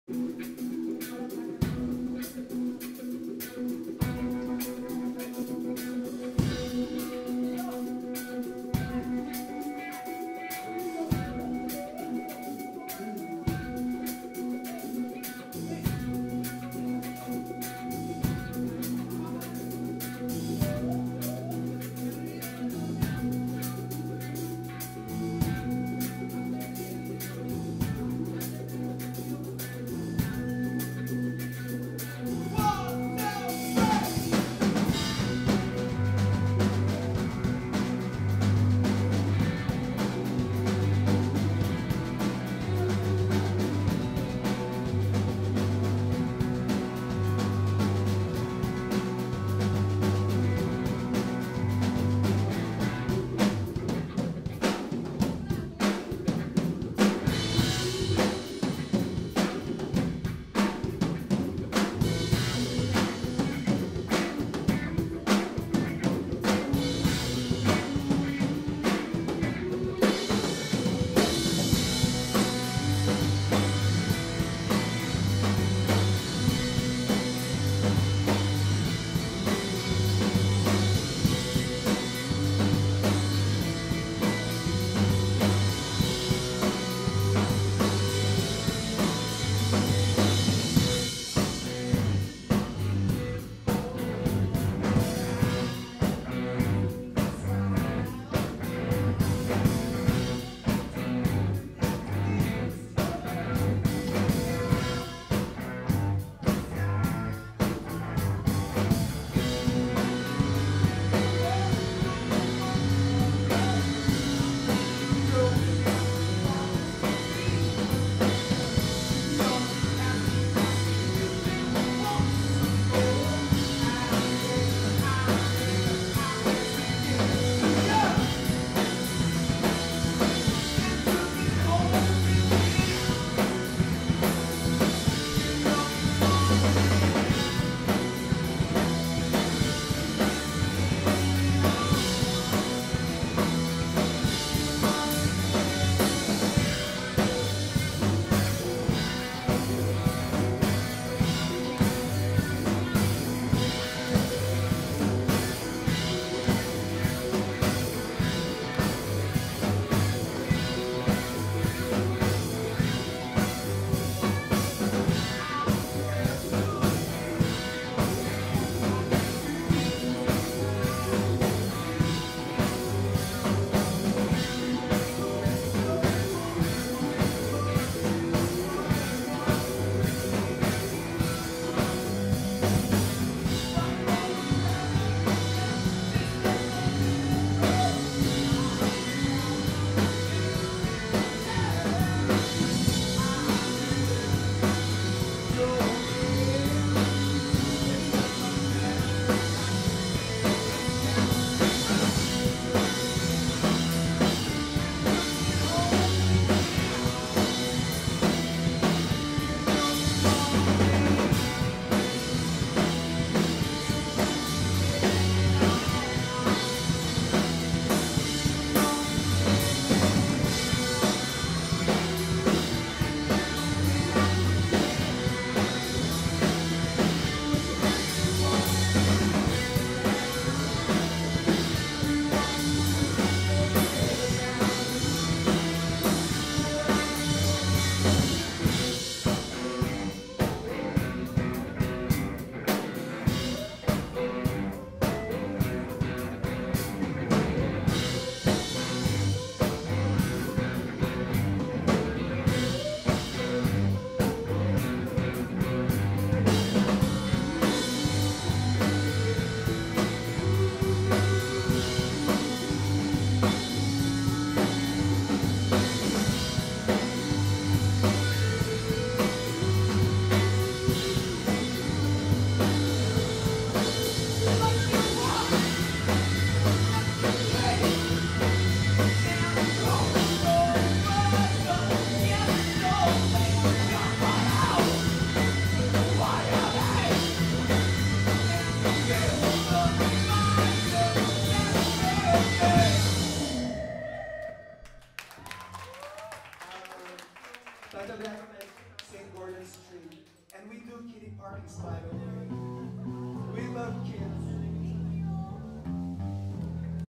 Tramonto live @ Sales Tekanplor
recorded with Zoom Q3HD, no post processing

house mic had really low volume